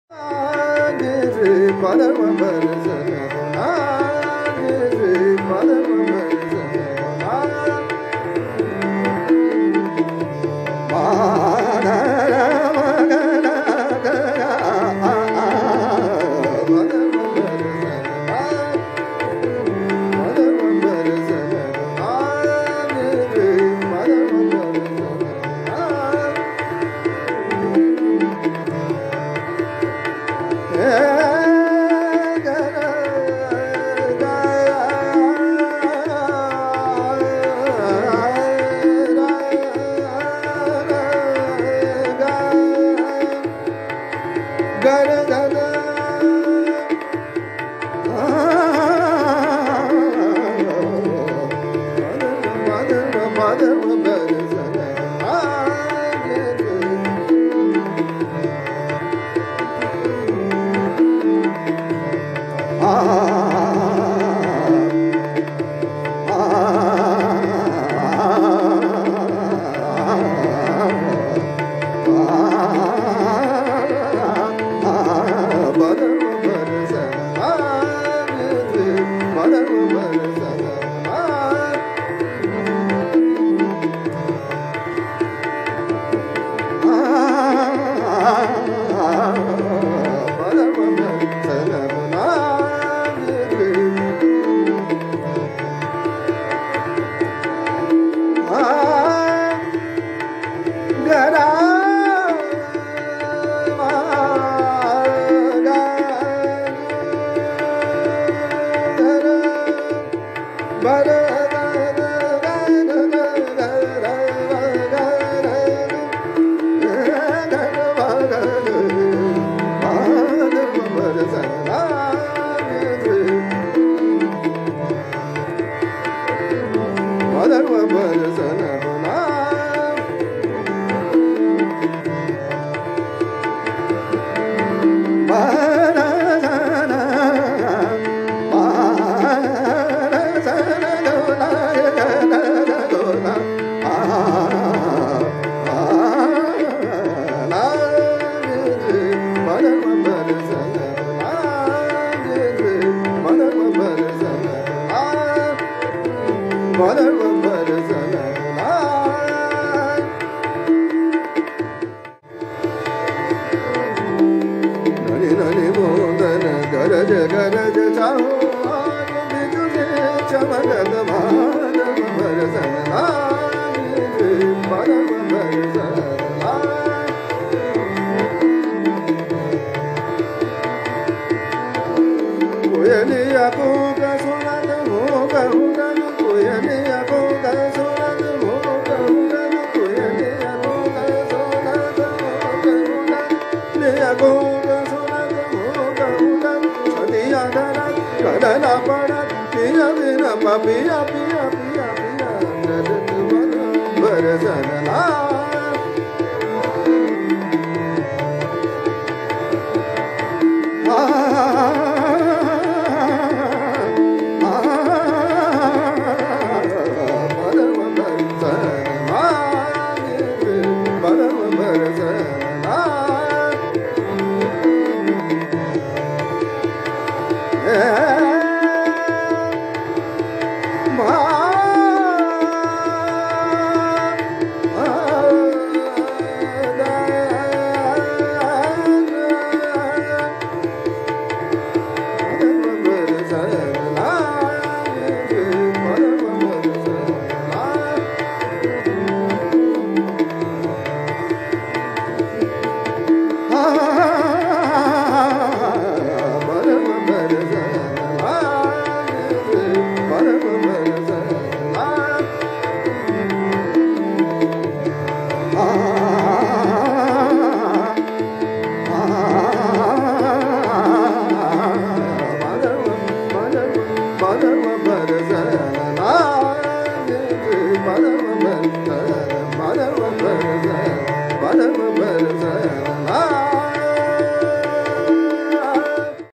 Bandish -Surmalhar drut.
A snippet of Rag Surmalhar drut Teental 16 beats composition.Vocal Subhankar Chatterjee